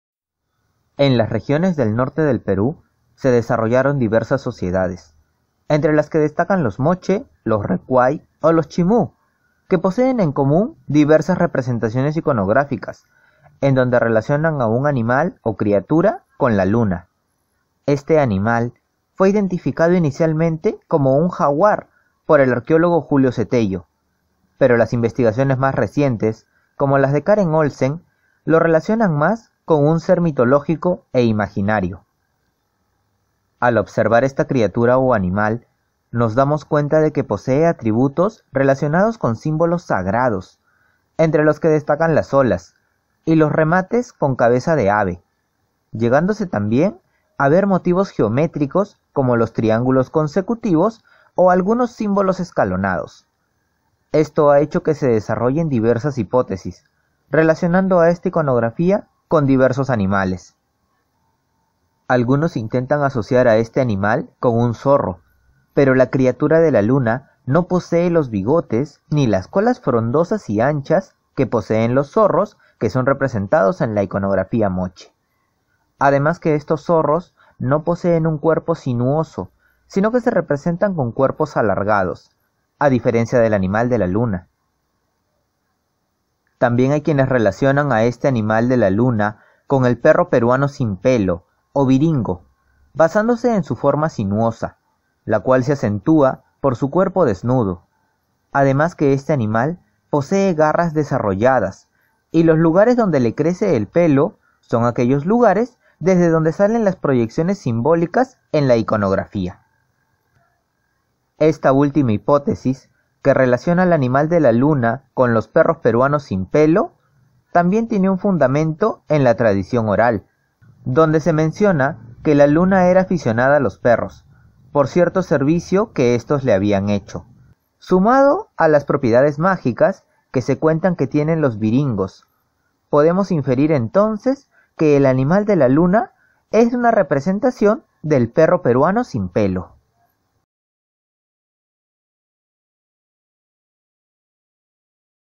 Animal de la luna
Existen muchas representaciones sobre animales en la iconografía prehispánica por lo que hoy trataremos el tema del animal de la luna que aparece en las regiones del norte del Perú.